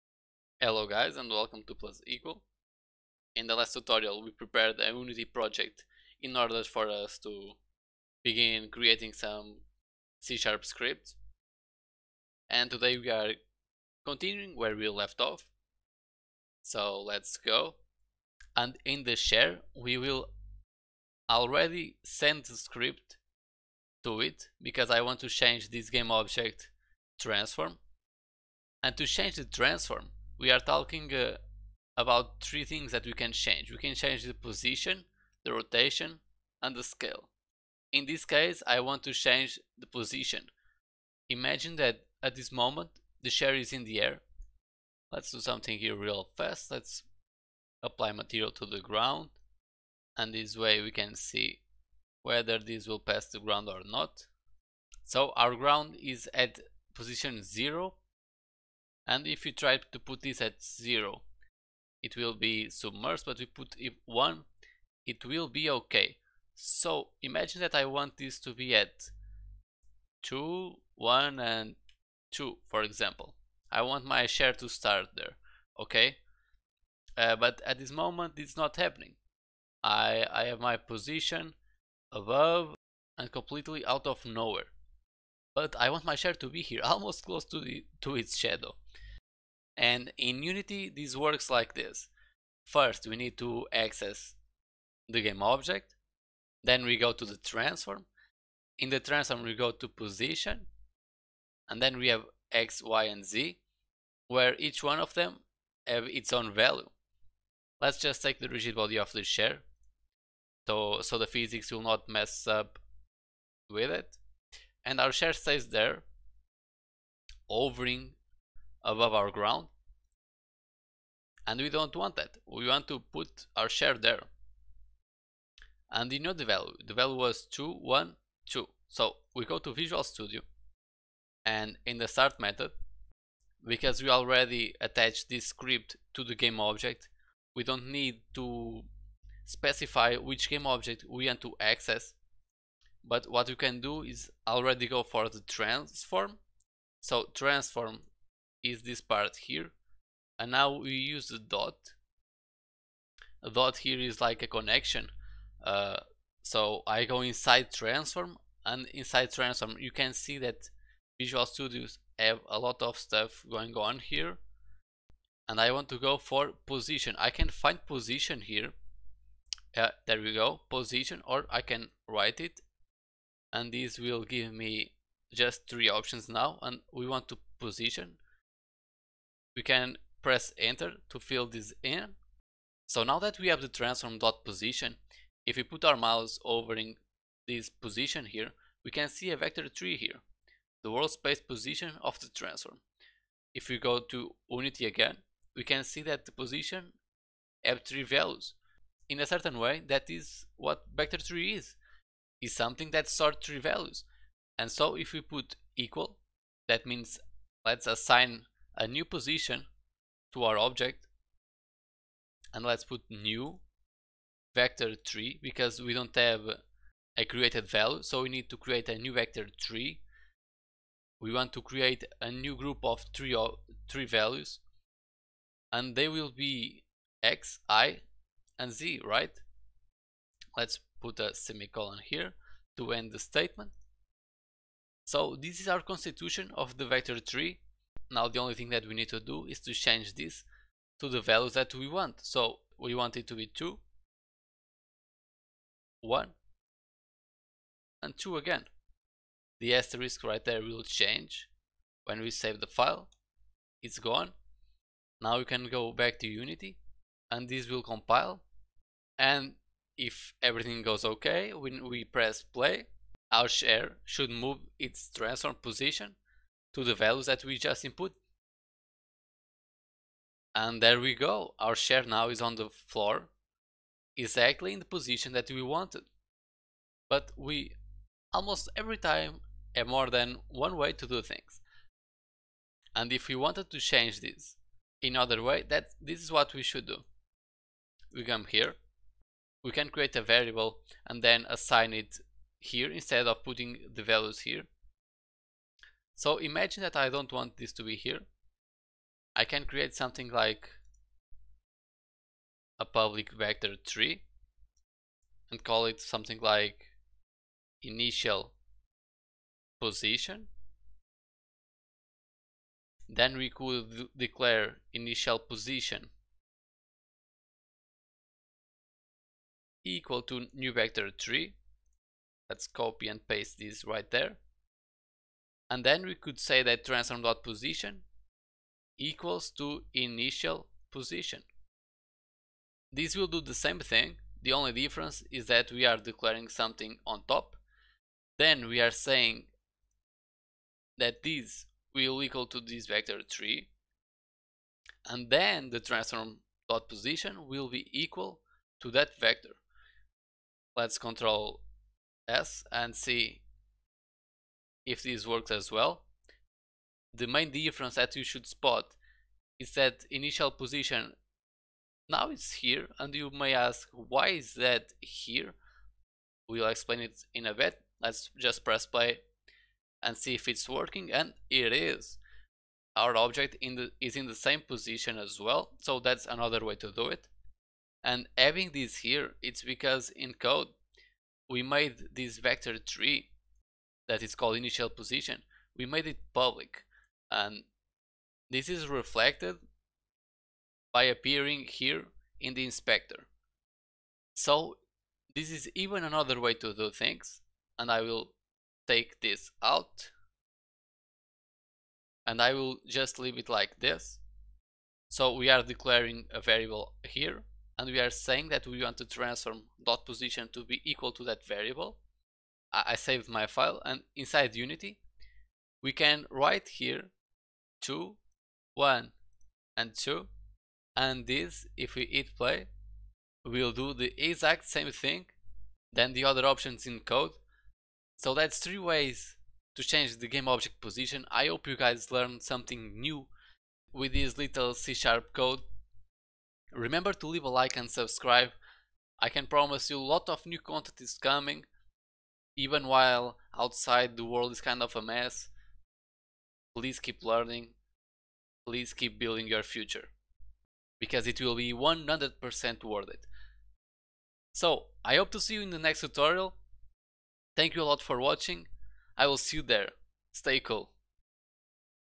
Learn to Code UNITY C# 2: Change the Game Object Position When Starting the Game ( 3 Different Ways)
In this short tutorial we will cover how to change an object position when pressing play in Unity, using the Start method, we also will cover what it means to use a Vector 3, and how to make it public so you can write the values for the position that you want, inside Unity, in the Game Object inspector.